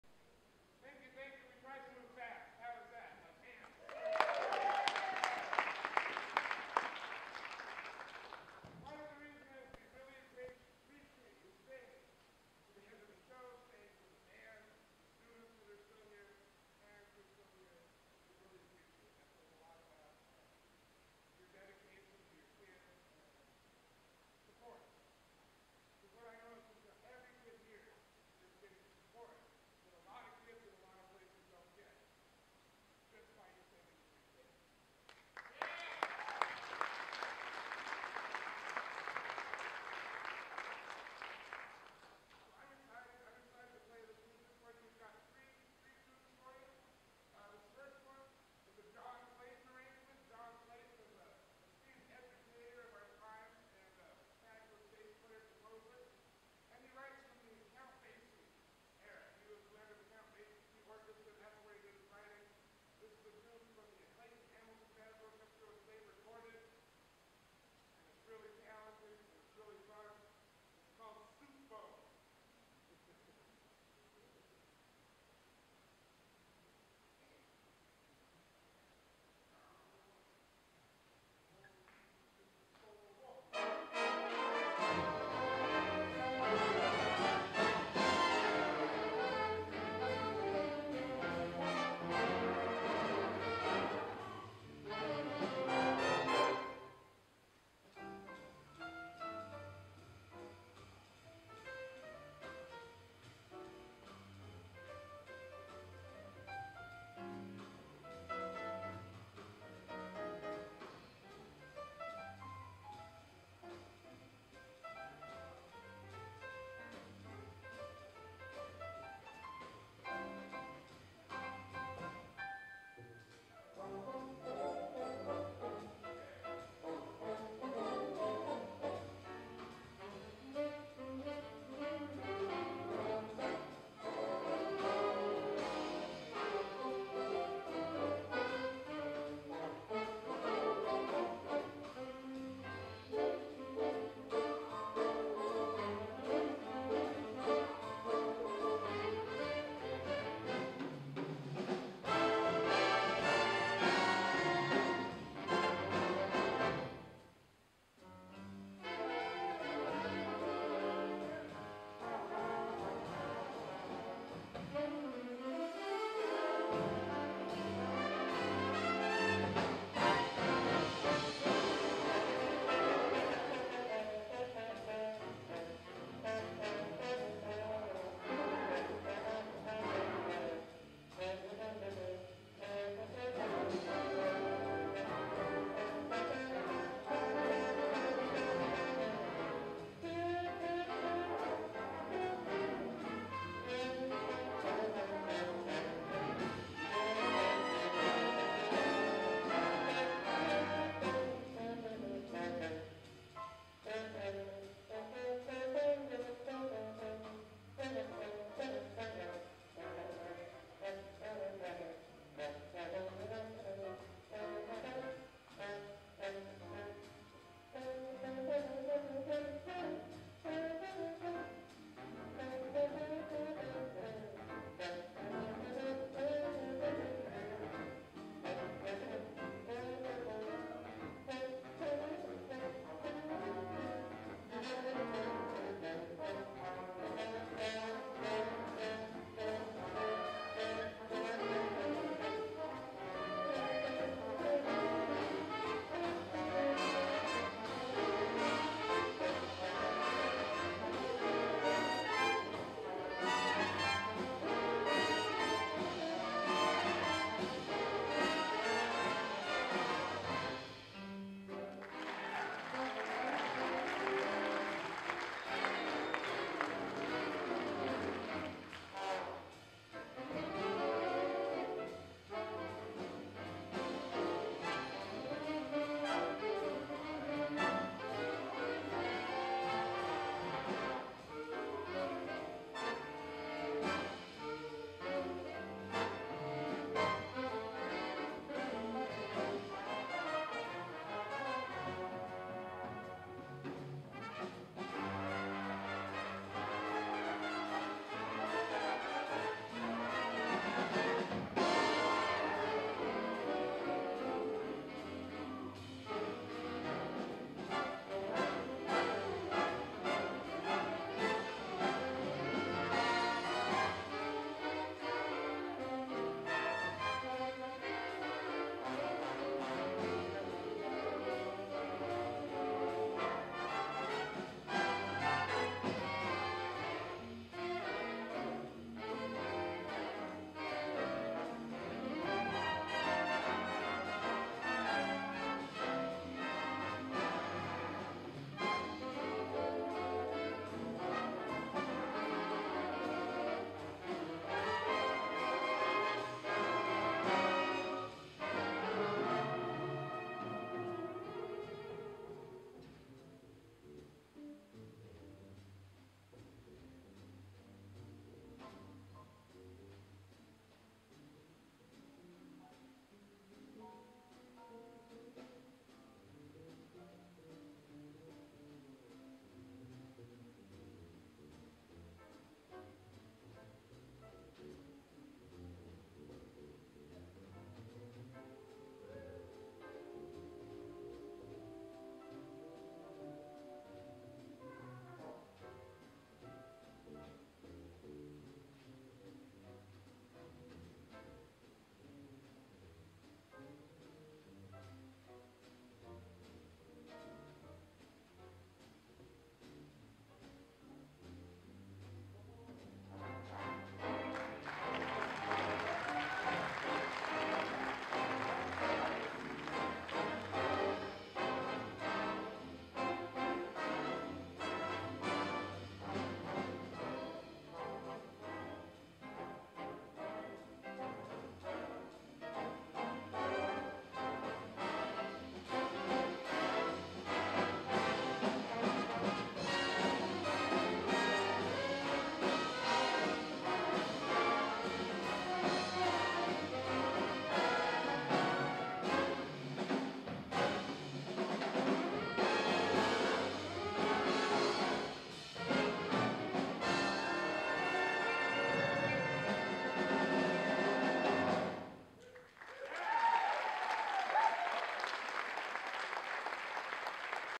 Robert's Big Band Part 1 of 2
Video of the Saturday performance of the 2012 Mel Brown Jazz Camp.

Shot with a Nikon D7000 & External Microphone.